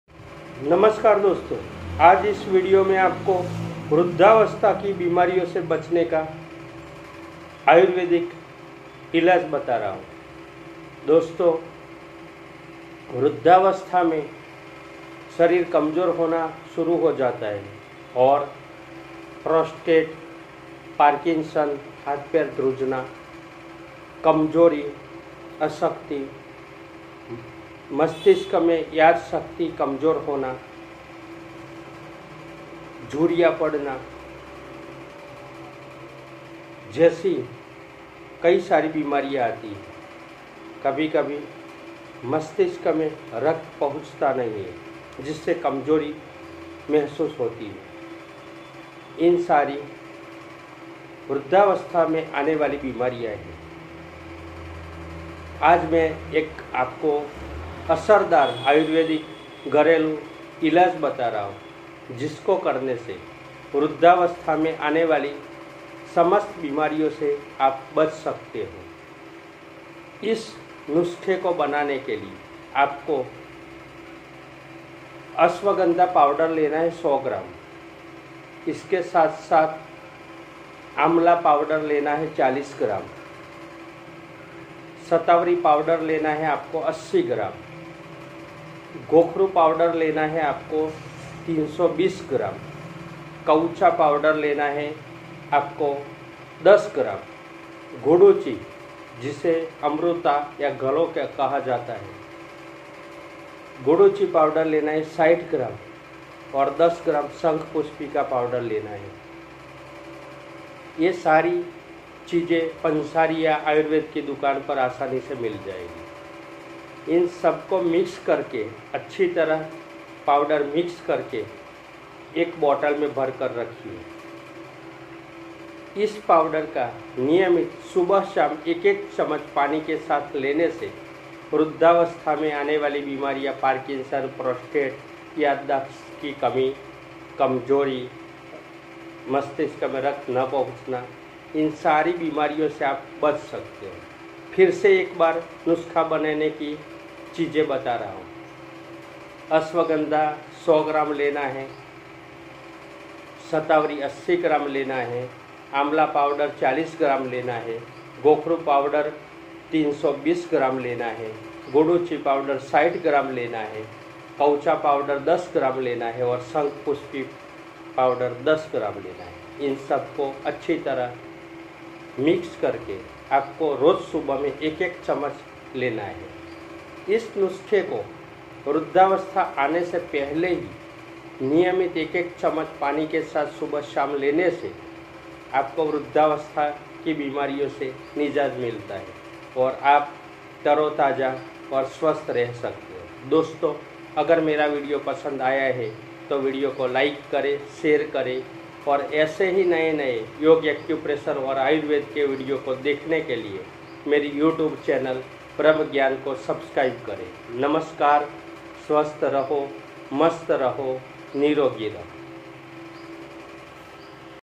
वृद्धावस्था के रोगो से बचने का उपाय। वृद्धावस्था के रोगो से बचने का आयुर्वेदिक उपाय । BHRAMGYAN
Hello,
I am Anil mistri. Welcome to our you tube channel “BHRAMGYAN.”

Me  is video me:- इस वीडियो में आप को वृद्धावस्था के रोगो से बचने का उपाय बताया है वृद्धावस्था के कारण आने वाले रोगो के बारे में बताया है साथ में वृद्धावस्था की बीमारियां से बचने का आयुर्वेदिक उपाय बताया है

आप विडियो पुरा अंत तक देखे। धन्यवाद

Thank you For watch Time